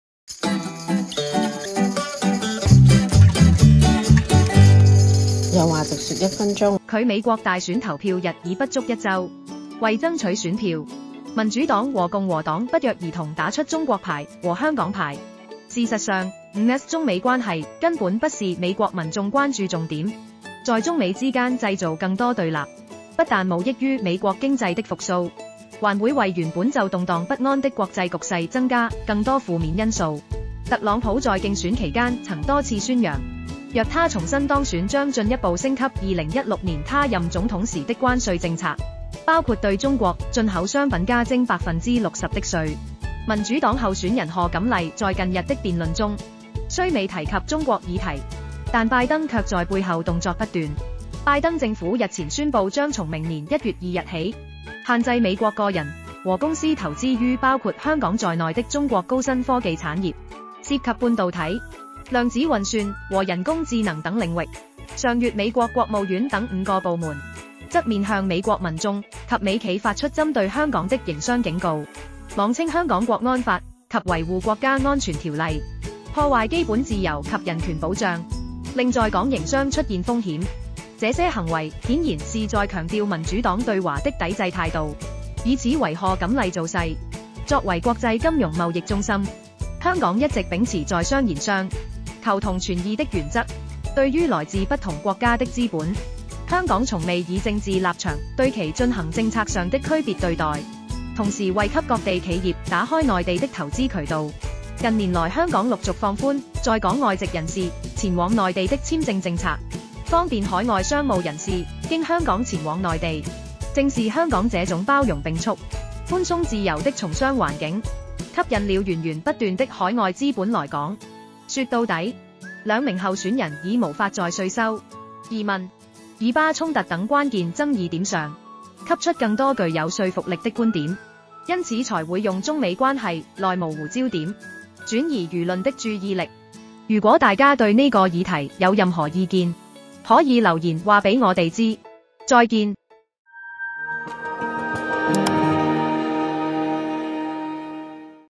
有話直說一分鐘｜打「政治牌」屬模糊焦點 經貿往來應在商言商
作為國際金融貿易中心，香港一直秉持在商言商、求同存異的原則。對於來自不同國家的資本，香港從未以政治立場對其進行政策上的區別對待。同時，為給各地企業打開內地的投資渠道，近年來香港陸續放寬在港外籍人士前往內地的簽證政策，方便海外商務人士經香港前往內地。正是香港這種包容並蓄、寬鬆自由的從商環境，吸引了源源不斷的海外資本來港。